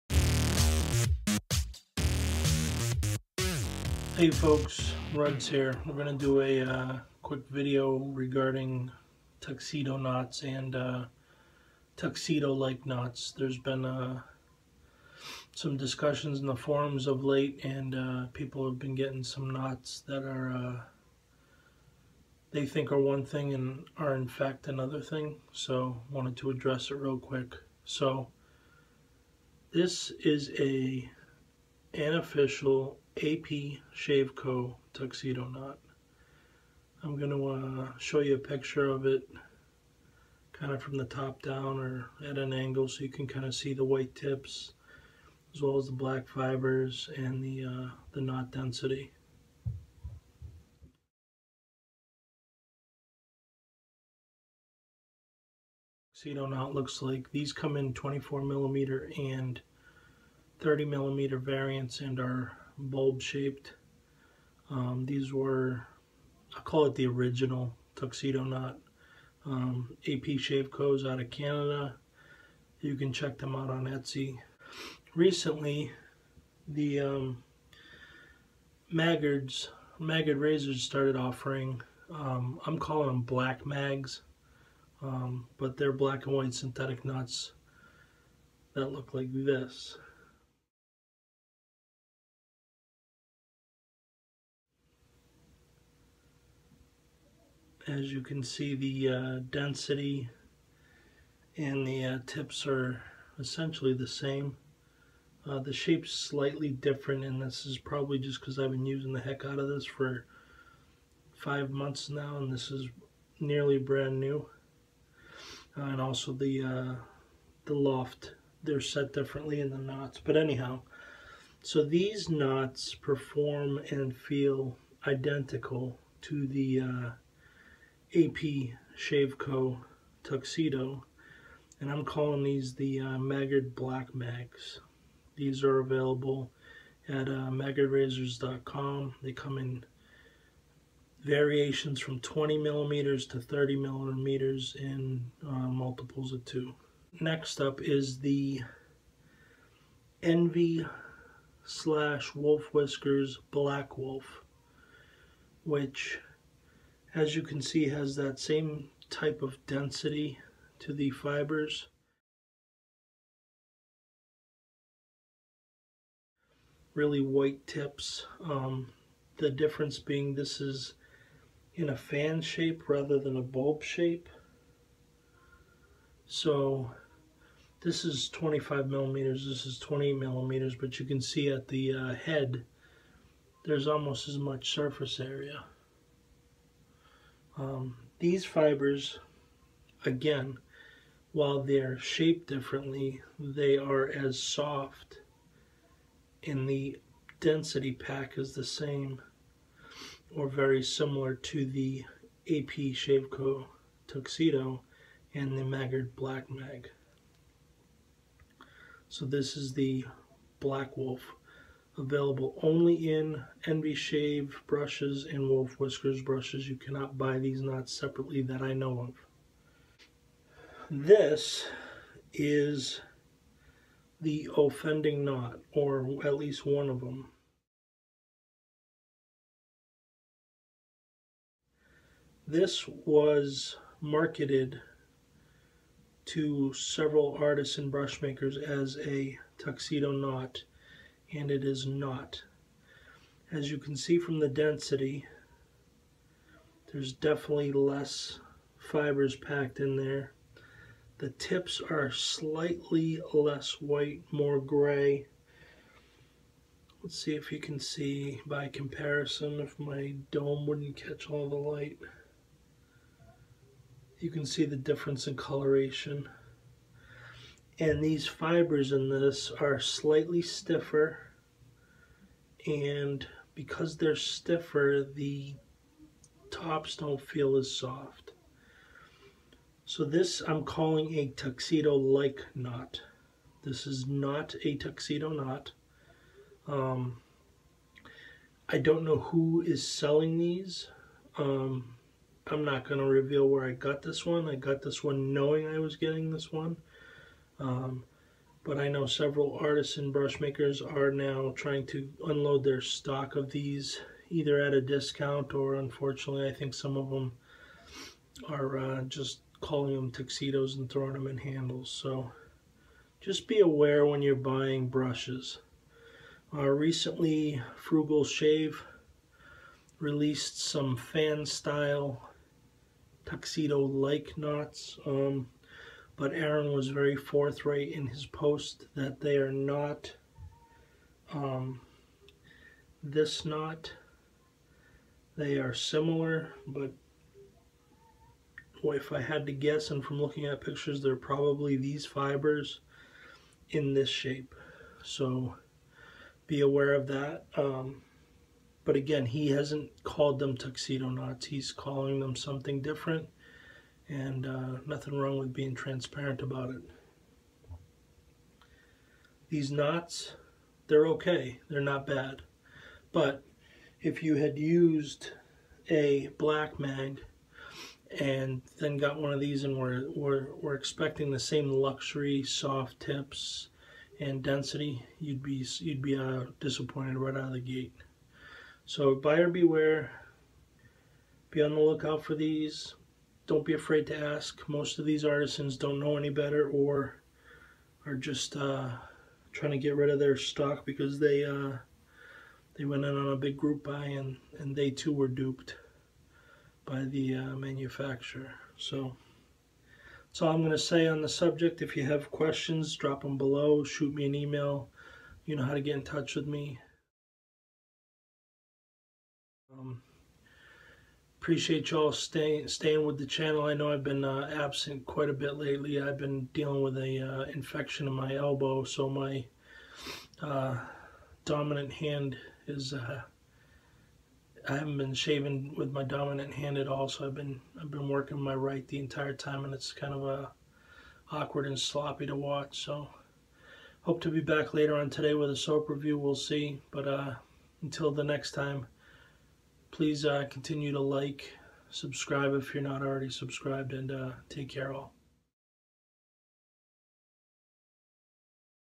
PSA - Tuxedo Knots and Knock Offs
So there has been an increasing number of buyers out there who have received knots from brush makers or knot distributors that are not actually “Tuxedo” knots. The term “Tuxedo” knot began with the original seller of these knots, APShaveCo. Around that same time, Envy Shave and Wolf Whiskers unveiled their fan style “Black Wolf”. Most recently, Maggard Razors released their own black and white synthetic knots, I am calling them “Black Mag”.

The Tuxedo, Black Mag and the Black Wolf are identical synthetic fibers but packed differently, therefore giving them different shapes. The Tuxedo is a bulb shape and the Black Wolf is a fan shape. They all have luxurious soft tips and a densely packed knot that provides ample backbone and splay resistance. I can recommend any one of these or all of these for use and enjoyment.

Recently, many artisan brush makers went in on a group buy for what they were told where Tuxedo knots. What was received was not a Tuxedo knot. They are what I call “Tuxedo like”. The fibers are not the same. They are slightly thicker which creates more splay residence and more backbone to the point where the tips feel less soft. They are also not as densely packed. While these knots are not awful, they do not provide the luxury that is associated with the term Tuxedo knot (or Black Mag or Black Wolf for that matter).

Be aware when working with artisan brush makers. Ask them about the knot supply, ask for photos, ask if they are aware of the differences and by all means show them this video.